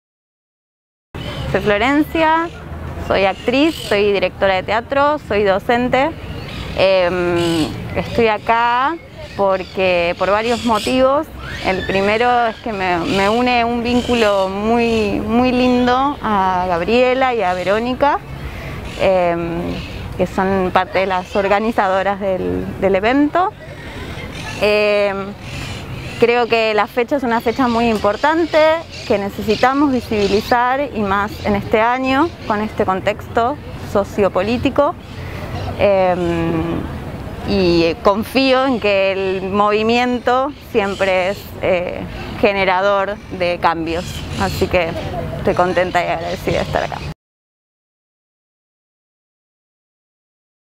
Microrelatos de Voces Colectivas surgidas de Acción en Danza.
🔻Nos mueve la transformación en vibración constante con la vida y el hacer artístico. Nos vinculamos y pensamos colectivamente, nos hacemos presente con lo que sentimos, deseamos, soñamos, creemos, creamos, trabajamos y luchamos por otra humanidad posible en un mundo sin violencias.

🔻Artistas participantes: performers, bailarinxs, estudiantes de artes del movimiento:
Sofía Pelliciaro - Laura Paolino - Virginia Ravenna - Alejandrina Lo Sasso - Beatriz Zuloaga - Candela
Pahihe - Anabella Ferreira - Emilse Ianni - Camila Reyes - Maria Jose Cadelago - Mayra Dómine - Andrea
Valeria Pagola - Sabrina Aylen Liscio - Camila Constanza Briones Ruggeri - Paola Alejandra González -
Florencia Soledad Sandulli - Pilar Gonzalez - Gabriela Montes - Pamela Nebbia - Lucía Herrera.

🔻EQUIPX
Dirección General: Ladys Gonzalez - Wanda López Trelles
Idea original: Ladys Gonzalez
Coordinadora de la puesta performática: Gabriela Romero
Composición sonora: Dúo Veza: Mayra Dómine/Pianista - Valeria Pagola/Cantante
Voz andante: Anita Tripodi Paz
Sonido: Gabriel Paterno
Producción: Victoria Demasi, Nicolás Di Tizio, Valeria Pertovt
Comunicación integral: Analía Tripodi Paz - Jimena Garri
Diseño: Melina Tocce
Gestión de contenidos para RRSS: Nicolás Di Tizio
Registros audiovisuales: Gabriela Abraham, Wanda López Trelles, Ladys Gonzalez
Drone: Sacha Sain